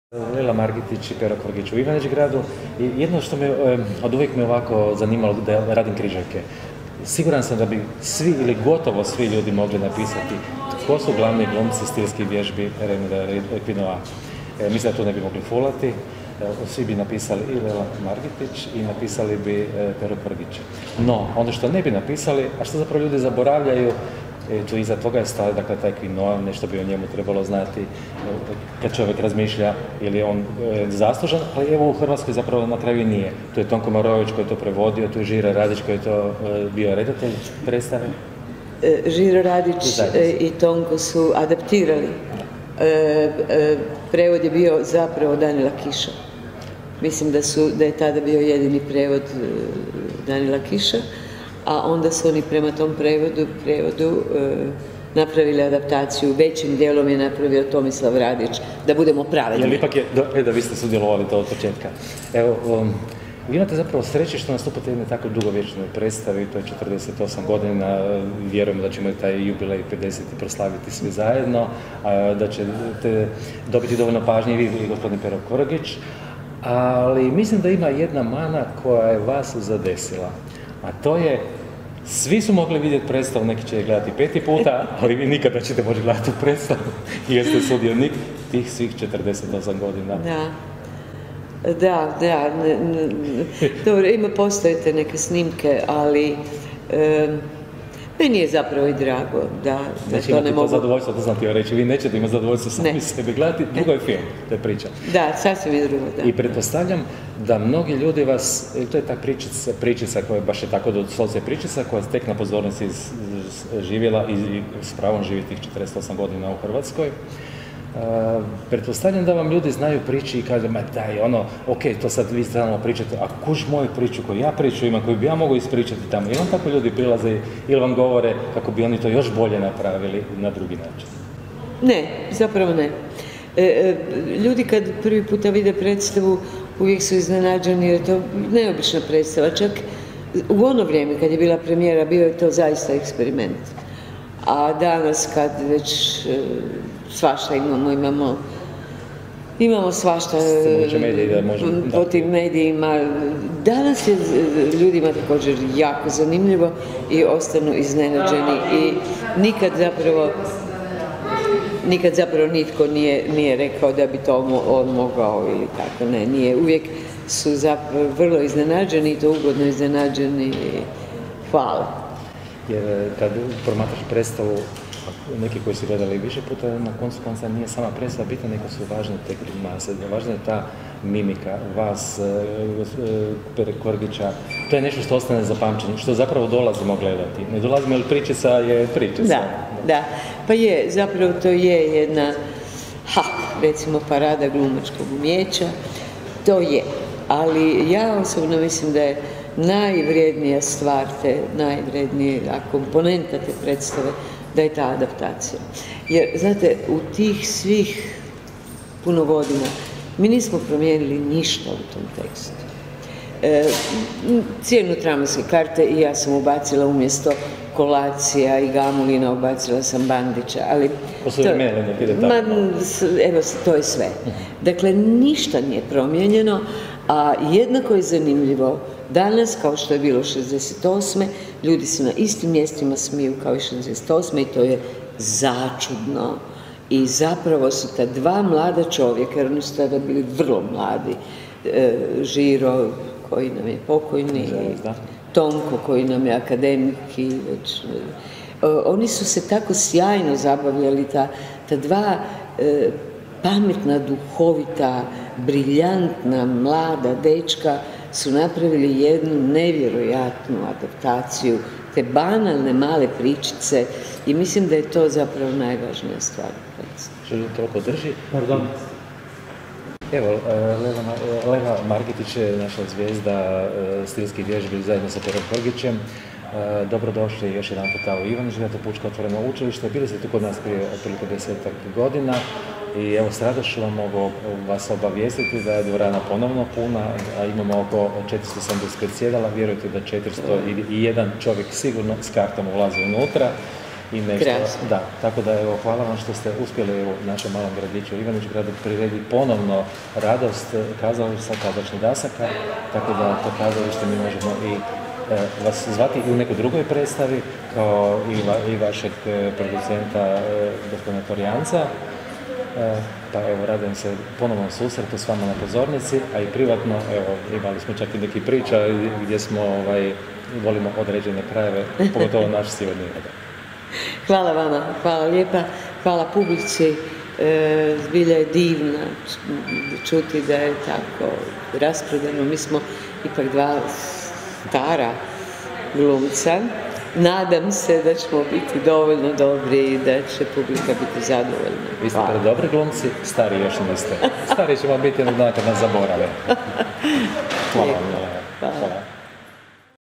Razgovor s Lelom Margitić
Prije ivanićgradske izvedbe najdugovječnije kazališne predstave na svijetu STILSKE VJEŽBE,  naš Jadranko Bitenc razgovarao je s legendarnom glumicom Lelom Margitić .

Predstava “Stilske vježbe” konstanta su u hrvatskom glumištu koja pokazuje da je kazalište uvijek zanimljivo, uzbudljivo i potrebno, a ona je i svjedok jednog od najboljih razdoblja hrvatskog kazališta i njegove kazališne tradicije.

Snimio: Siniša Vuger